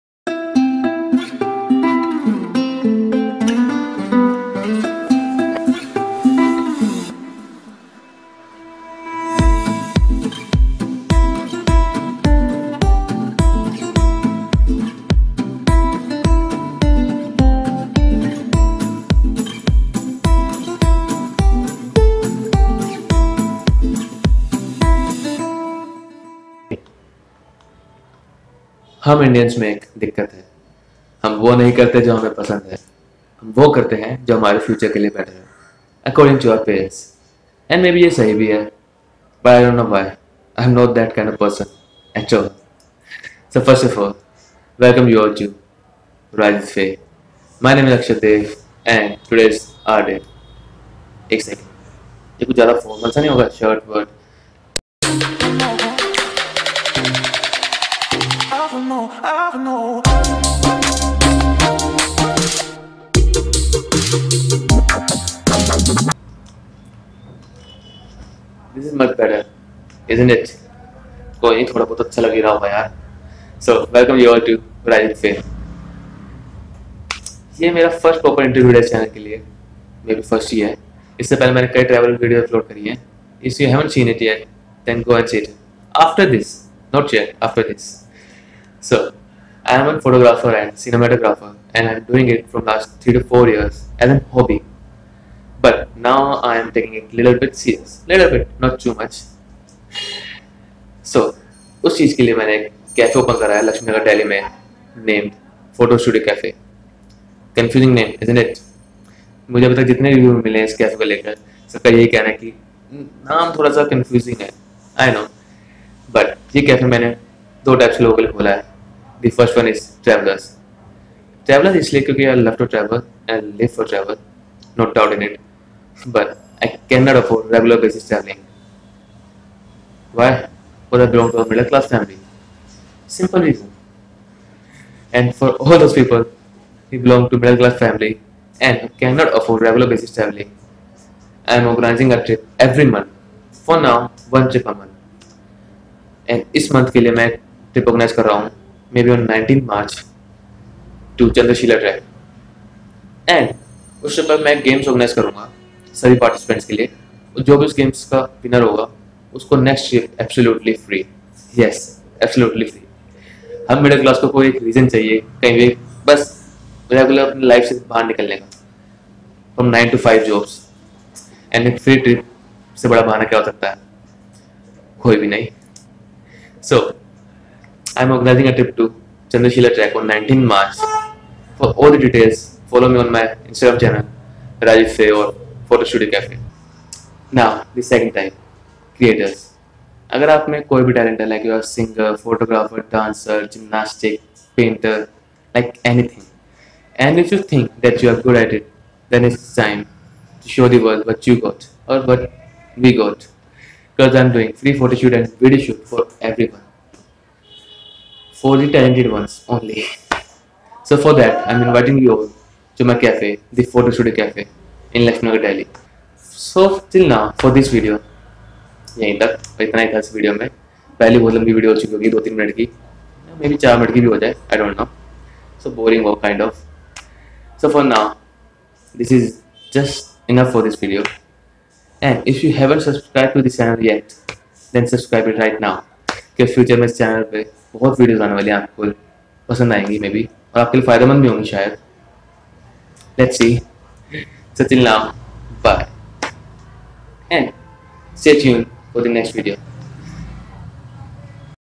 FREE TRIP & FREE PHOTOSHOOT !!!! PHOTO STUDIO CAFE | DELHI | INDIA
Hey Everyone !!
Welcome all of you to my channel Rise With Fay.
This video is about my Cafe that i opened in Laxmi Nagar, Delhi with a goal to live my life on my own terms and to help my self or others to grow with the talent that they have cause as an photographer i am inviting all the talented people to my cafe to collaborate with me and show the world what we got.
Also this video has a surprise for the Travelers who live for travel. So, just found it by watching it.